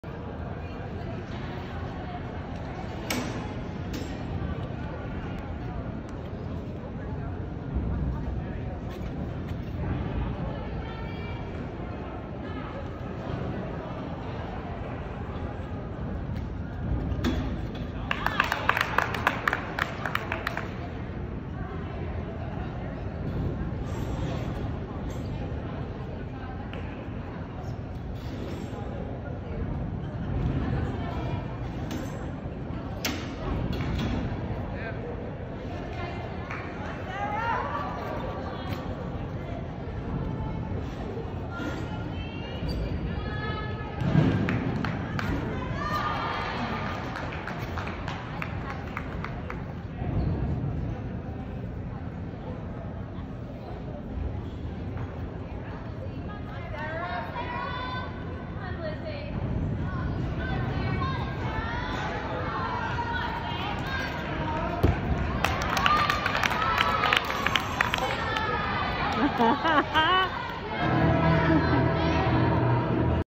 2021 Eastern Nationals- Level 9 Beam. 9.075–Class of 2023, Lanier’s Gymnastics
Eastern Nationals- Level 9 Beam. 9.075–Class of 2023, Lanier’s Gymnastics. Coaches Cindy Wallace & Amanda Ziegler